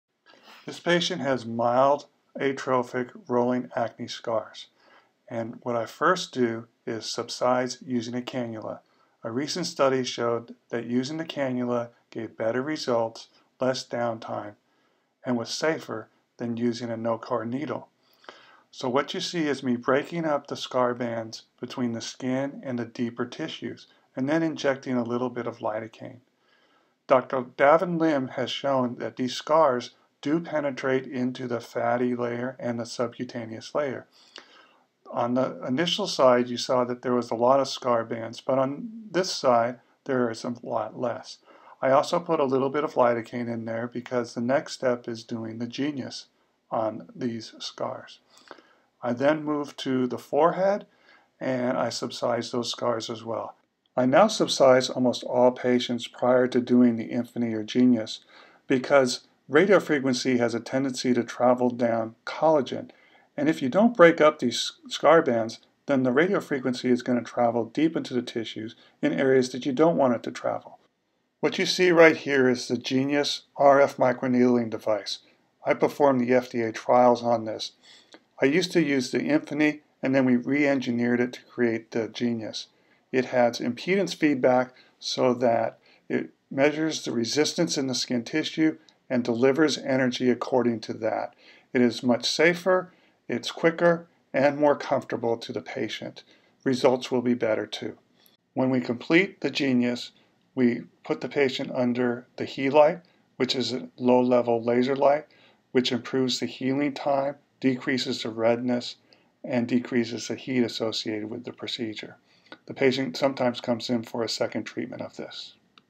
Rolling Acne Scars treated with Cannula Subcision, NEW Genius, and Healite
Dr. Weiner is showing how he treats mild rolling acne scars. His first step is to perform subcision using a 23g softfil cannula. The video demonstrates the tethering of the scars to the subcutaneous tissue and superficial fat layer, which is divided using the cannula. A small amount of diluted Lidocaine is placed in this layer to improve comfort with the Genius which follows the subcision. RF has a predilection to travel along collagen bands, so Dr. Weiner feels it is important to subcise these diving scar bands to prevent RF from traveling deeper than the intended area.
The Genius is a totally reengineered RF microneedling device, which has impedance feedback. This allows for better/more precise delivery of energy into the dermis. The needles and motor upgrades overcome the resistance of the tissues, so needle depth is more accurate. There is also an energy output display so energy per pulse and total energy is available to the operator. All these improvements lead to more comfort and a better overall outcome.
After all laser and RF procedures, patients undergo a Healite treatment. This is a LLLT (Low Level Laser Light Treatment) that improves healing time, decreases risk of complications, and improves outcomes.